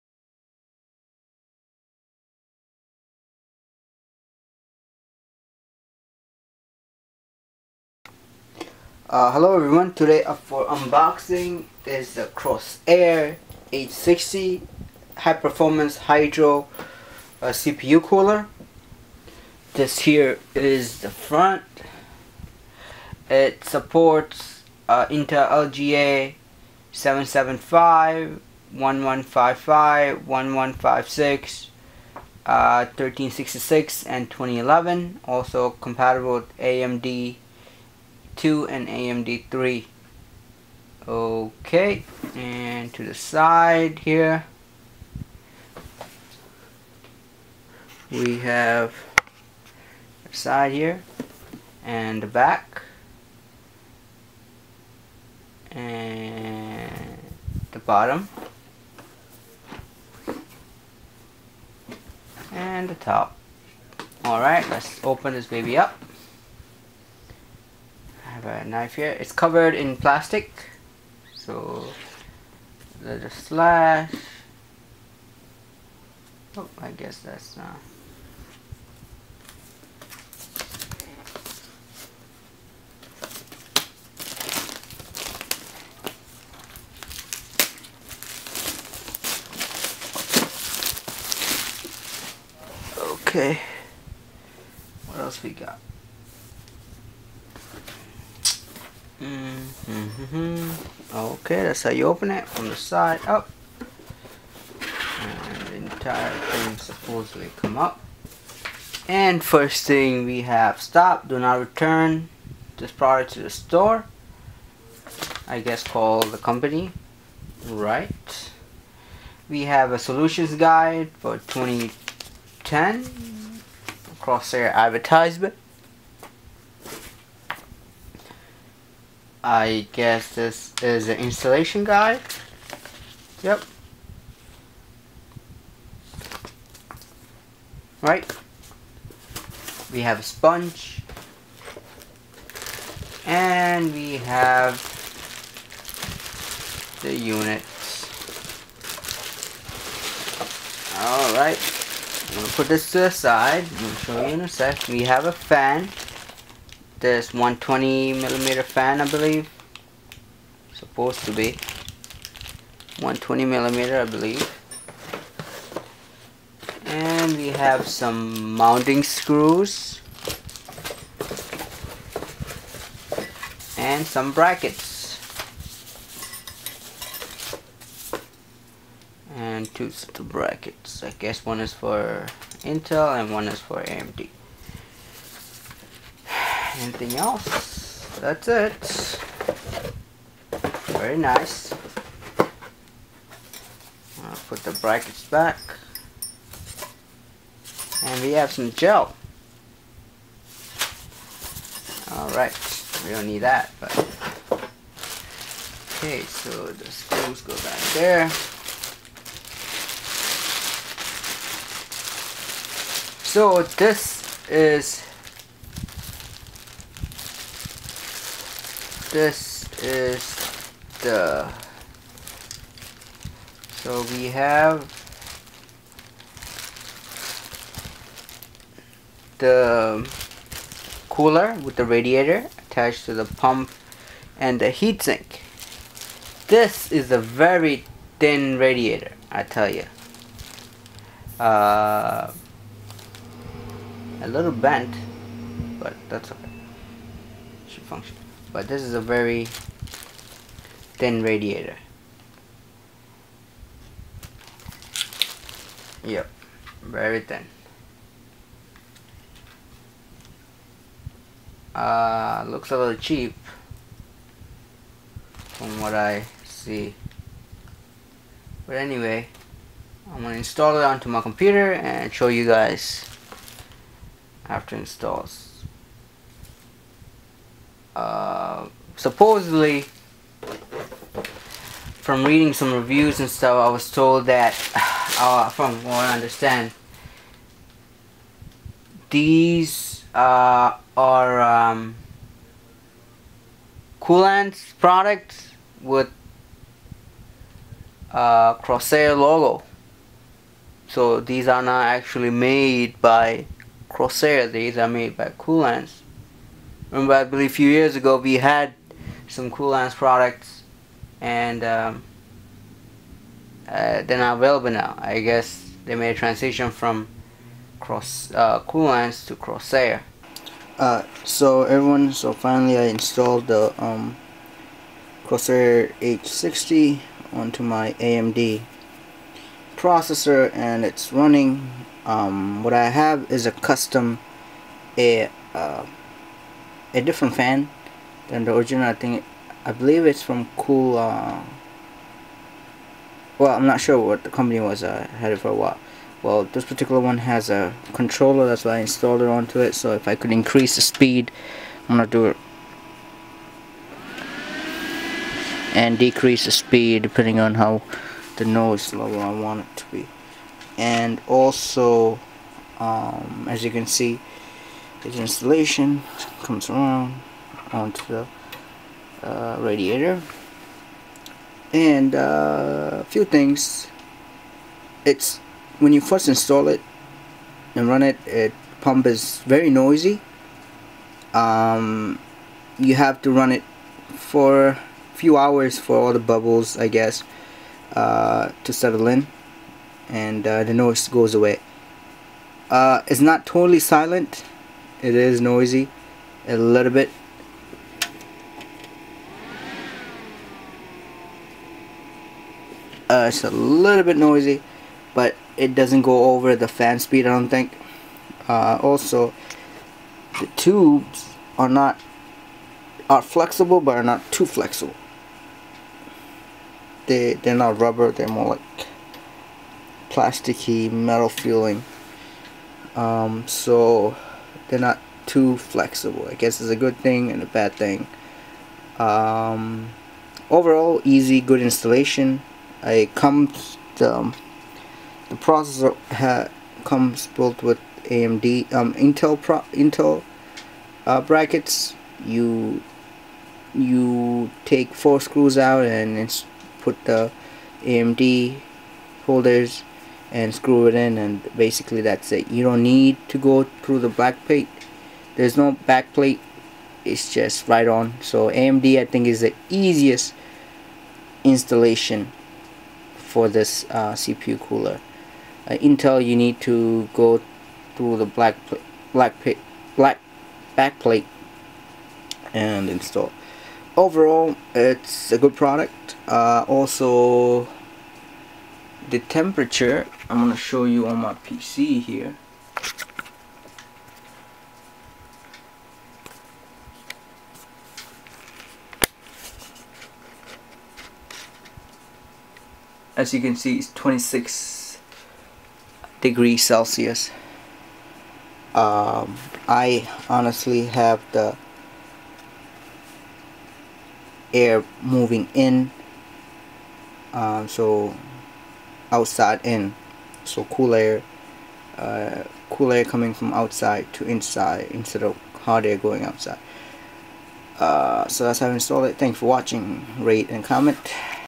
CORSAIR Hydro Series H60 High Performance Liquid CPU Cooler (CWCH60)
The Corsair Hydro Series H60 is a all in one water cooler for you CPU.